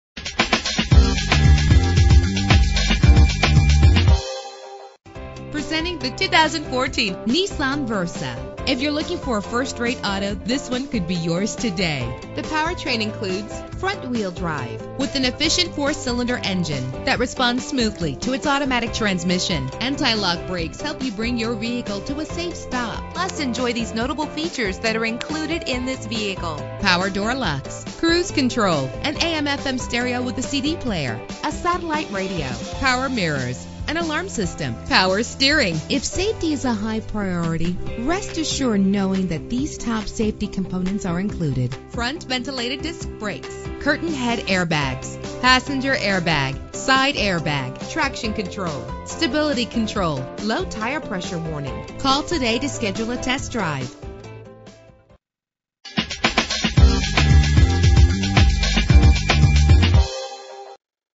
2014 Nissan Versa - Avenel New Jersey
Year: 2014
Make: Nissan
Model: Versa
Trim: 1.6 SL
Engine: 1.6 liter inline 4 cylinder 16 valve
Transmission: Automatic CVT
Color: Fresh Powder
Mileage: 0
Address:
90-100 Route 1 North